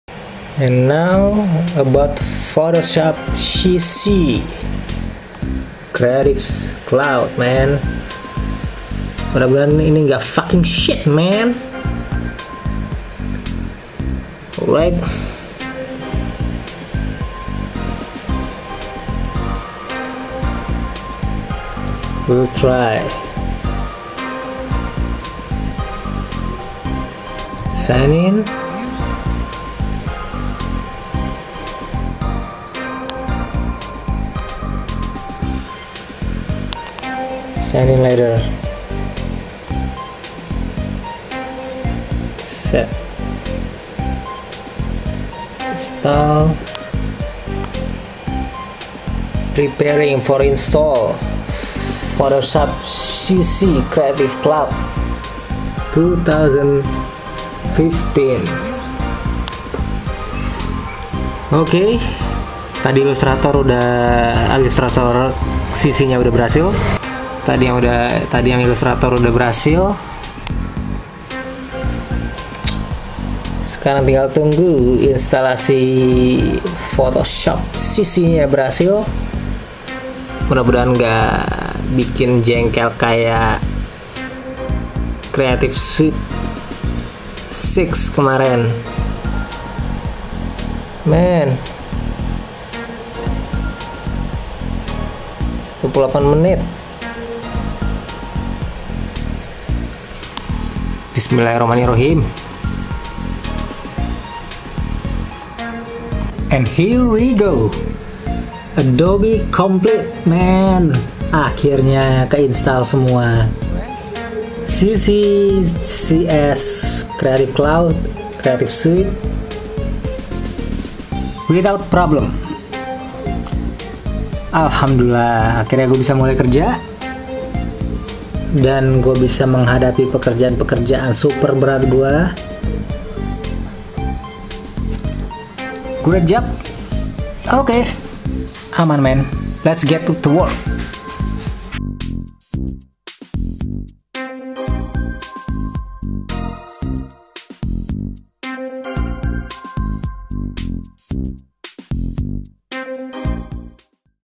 Di Balik Layar No.8 Finall Saga Of Installation Adobe CC.Hope the Sh...
Finaly!!!
Final Chapter of Shitman Part Final...
akhirnya instalasinya beres,..2017  beres, dan akhirnya pake 2 version...
Creative Suit & Creative Cloud.
asiikkk..
let's go to Work